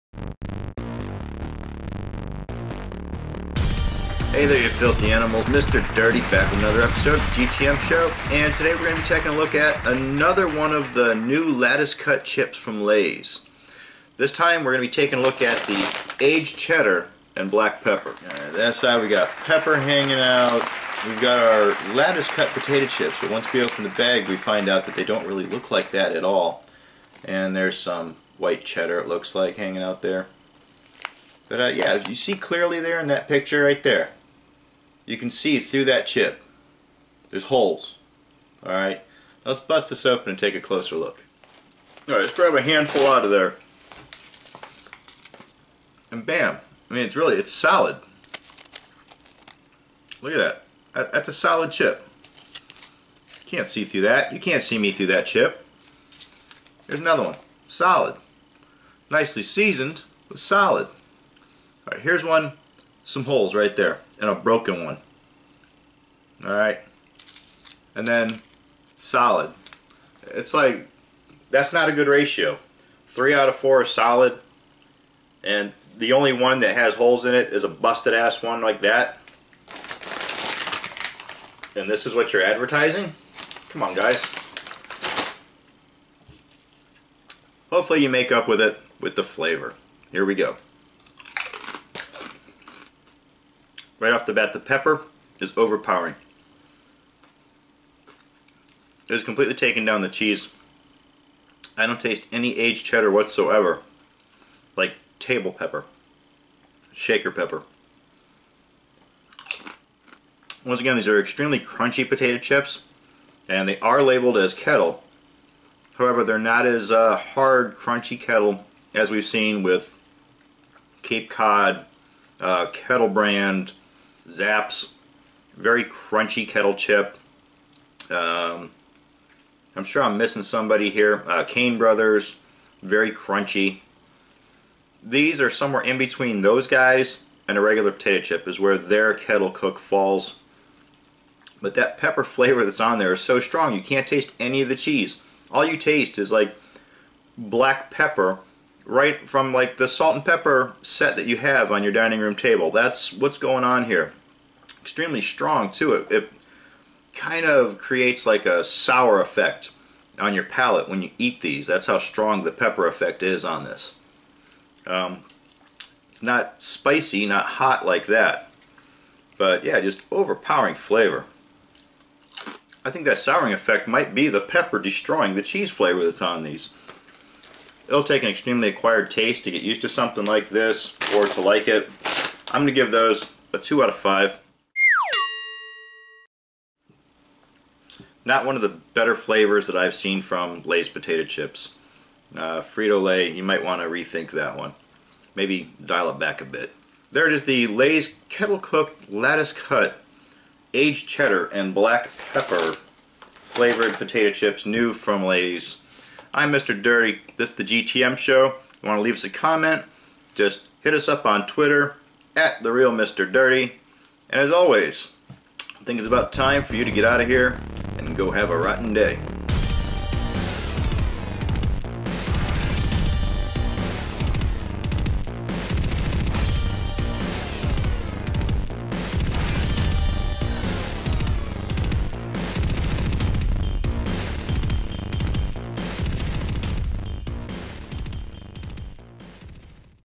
The GTM? Show - Lays Lattice Cut Aged Cheddar & Black Pepper
On this episode Mr. Dirty reviews the Aged Cheddar & black Pepper flavored Lattice Cut Kettle Cooked Lay's Potato Chips.